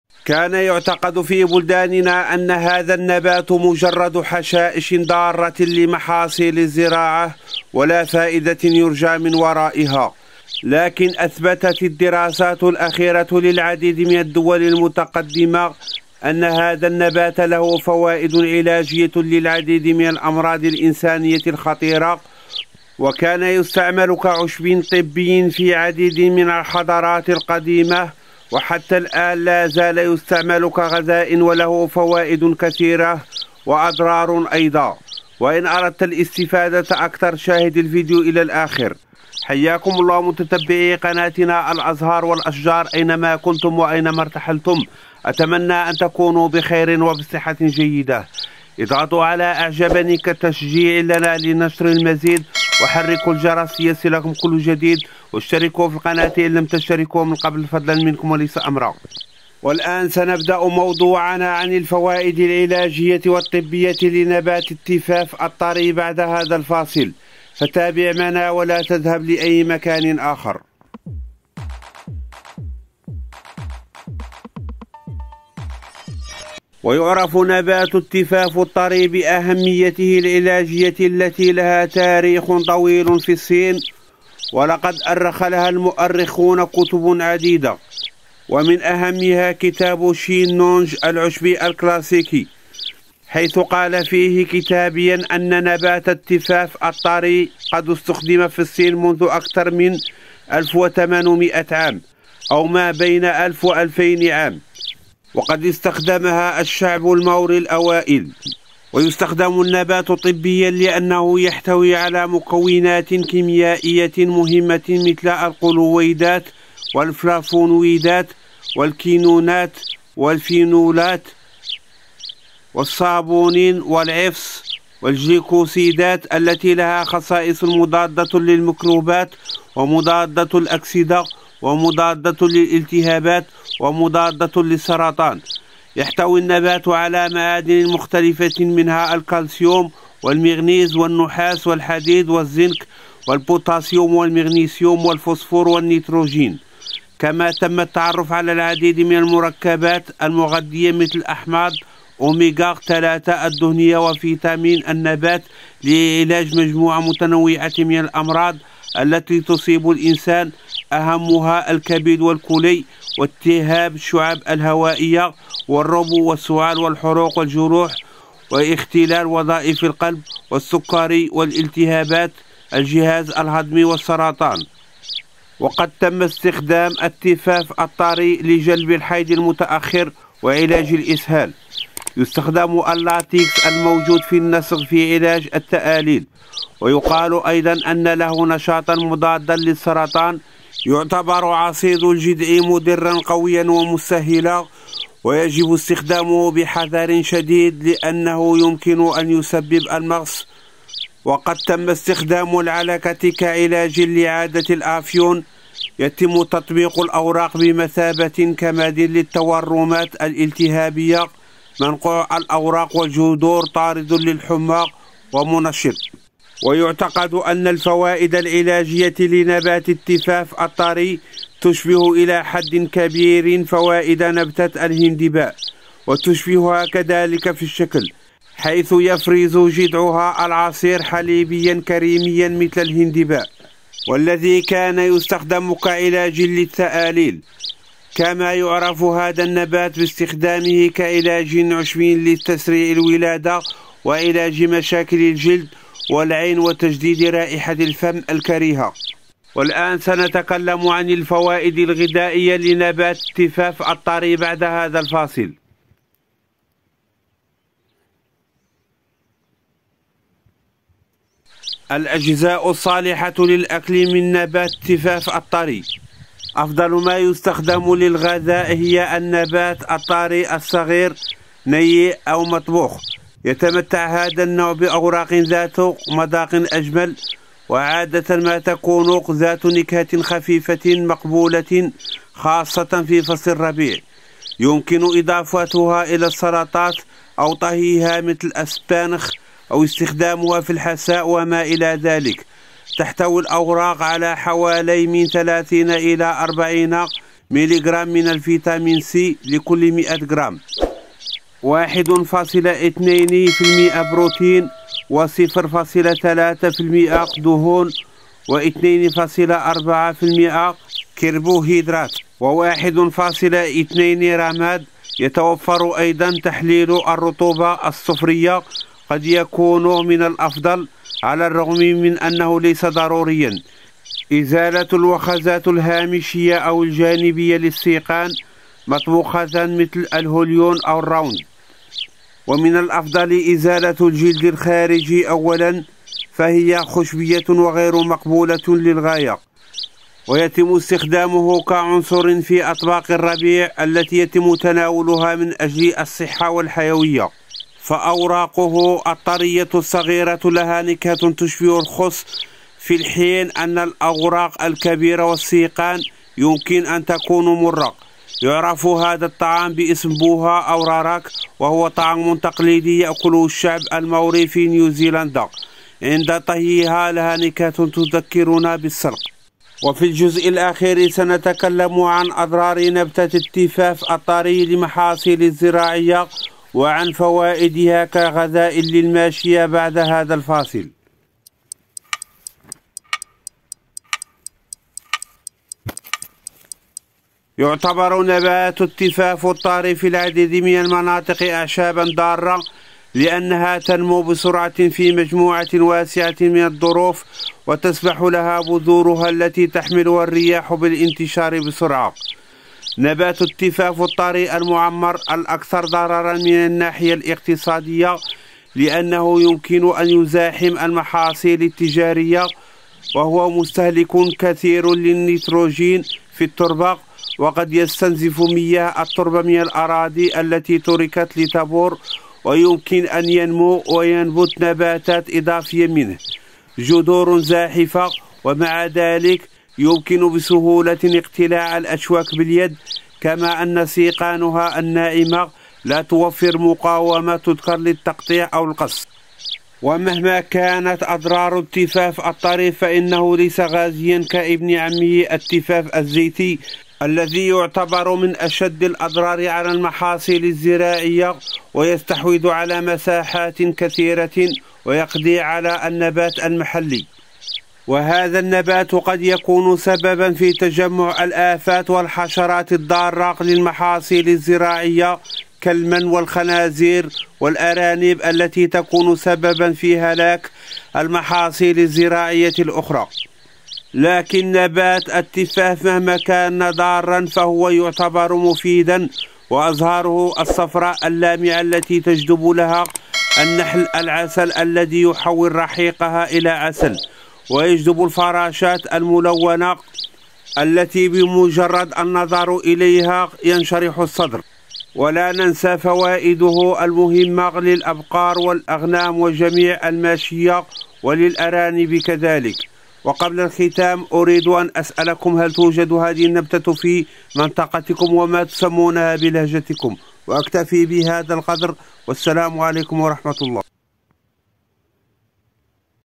نبات التفاف الطري واستخداماته العلاجية والإقتصادية التاريخية / Sonchus tenerrimus
نبات التفاف الطري واستخداماته العلاجية والإقتصادية التاريخية. يزرع في الحدائق ، جمال أزهاره ، إرتبط هذا النبات تاريخياً بصابورة السفن. وهي المياه التي تحافظ على اتزان السفينة في المناطق الساحلية ، يلقح بواسطة الحشرات ، يستخدم للعلاج ، يستخدم للأكل ، يستخدم في الوجبات الغذائية ، نبات يستعمل في الصين ، نبات يلقح بواسطة النحل والفراشات والحشرات الملحقة. ويعرف بإسم التفاف الواهي ، تفاف طري ، تفاف واهي ، التفاف ، الحوى ، الأثيوريزة وإسمه العلمي Sonchus tenerrimus. من الفصيلة النجمية ، الفصيلة المركبة ،